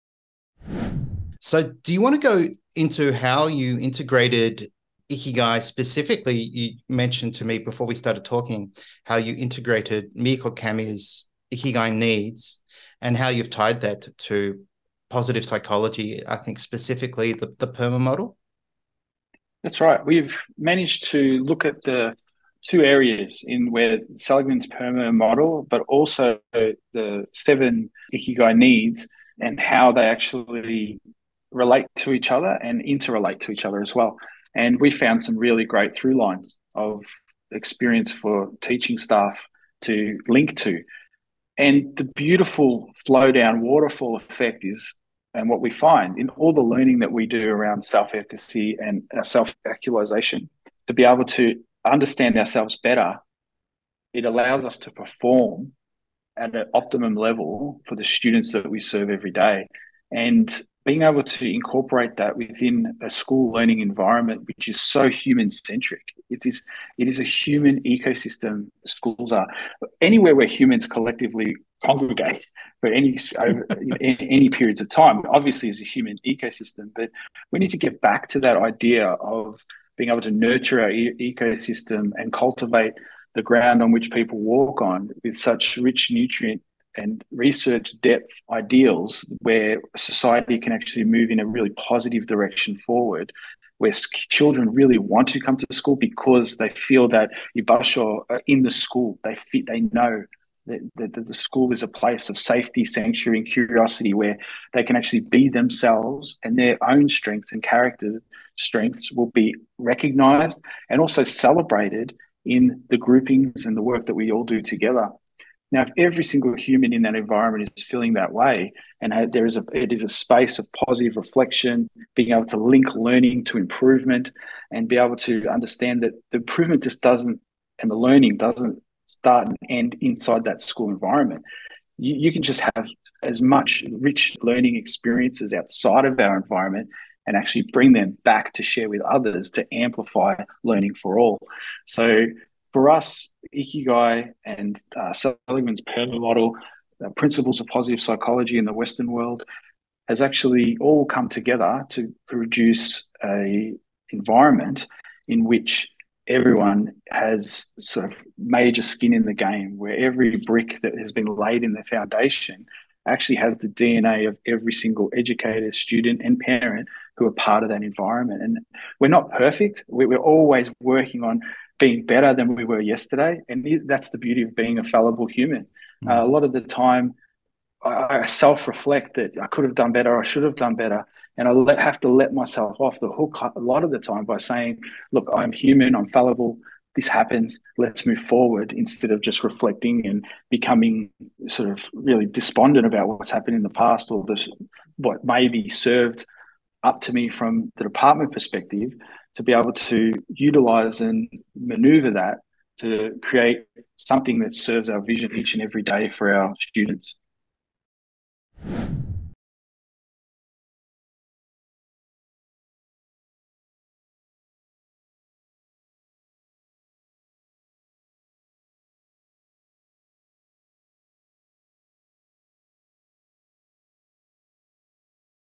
Applying Positive Psychology in Learning Environments
Matthew Borg and his colleagues were able to incorporate Mieko Kamiya's ikigai needs into their workplace culture. They related Kamiya's needs to Seligman's PERMA Theory, and used them as tools that have helped them understand themselves better, enabling them to perform at an optimum level for their students.